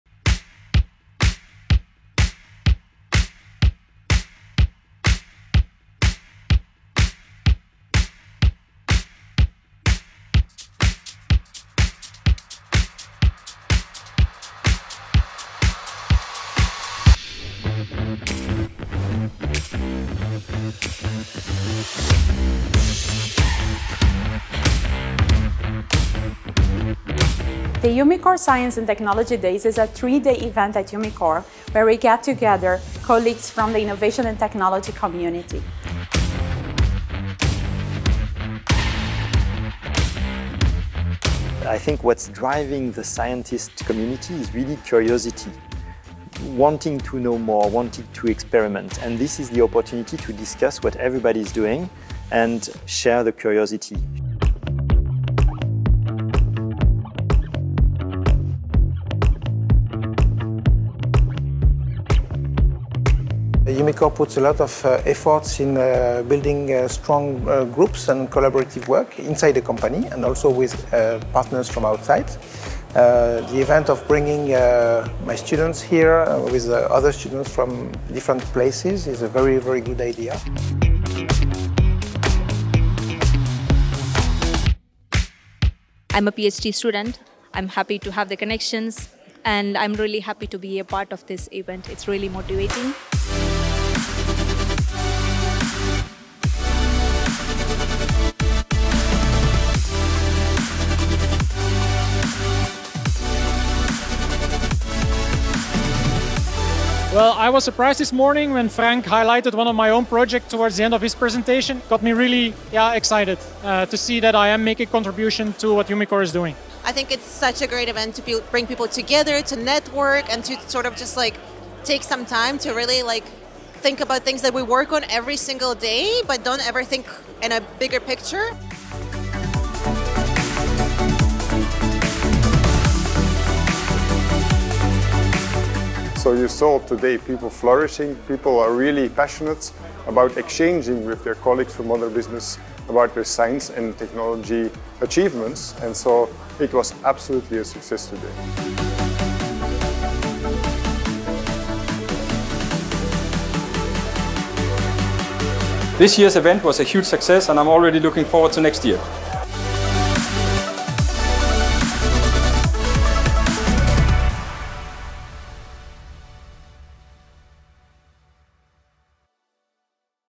Umicore Science & Technology Days
Can you imagine a day filled with updates on the latest discoveries from the whole science and technology community at Umicore? That's what the Science and Technology Days are! Sharing curiosity, findings and promoting collaboration, strengthen our position to be a leader in innovation and technology. Curious for more? Watch a sneak peak of our aftermovie!